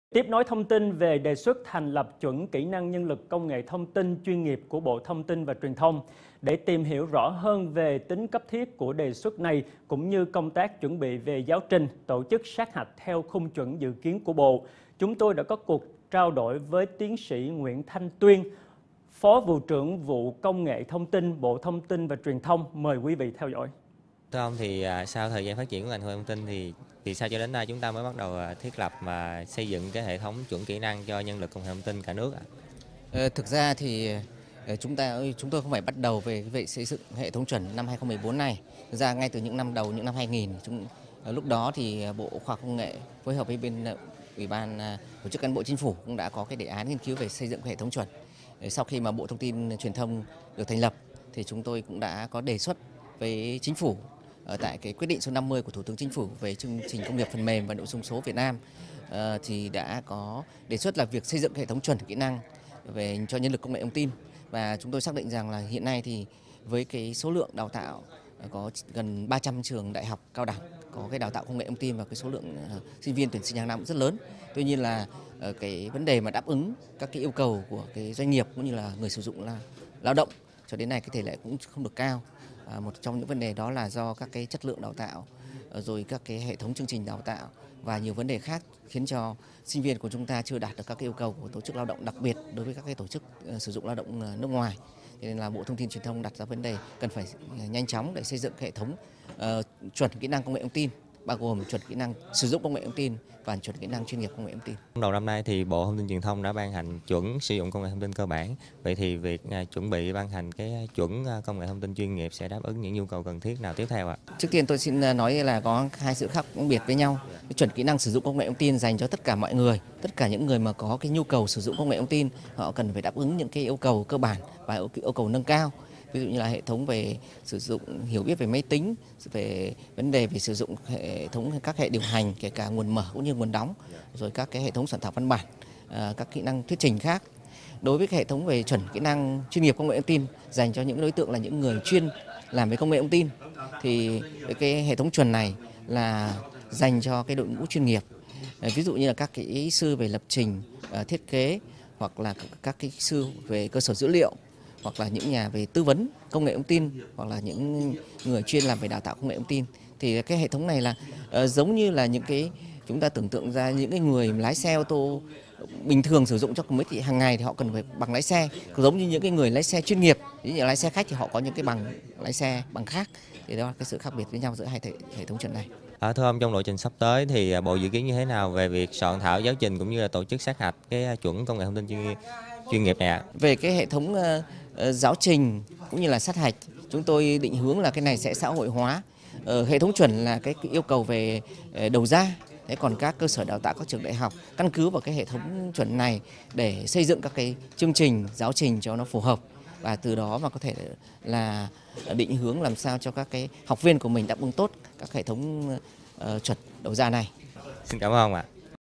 Bộ TT&TT đã chuẩn bị gì cho đề xuất lập chuẩn kỹ năng CNTT?
Tiếp nối thông tin về đề xuất thành lập chuẩn kỹ năng nhân lực công nghệ thông tin chuyên nghiệp của Bộ thông tin và Truyền thông. Để tìm hiểu rõ hơn về tính cấp thiết của đề xuất này cũng như công tác chuẩn bị về giáo trình, tổ chức sát hạch theo khung chuẩn dự kiến của bộ, chúng tôi đã có cuộc trao đổi  với Tiến sĩ Nguyễn Thanh Tuyên - Phó vụ trưởng Vụ Công nghệ thông tin - Bộ Thông tin và Truyền thông , mời quý vị theo dõi.